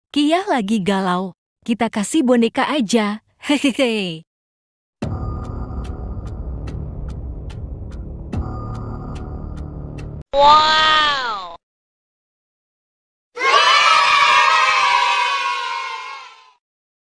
Parodi Lucu‼️Qiyah Lagi Galau...‼️Sulap Menggerakkan Boneka || Hanya Pakai Hp
Qiyah Lagi Galau...‼️Terpaksa Kita Buka Magic Lagi...😂😂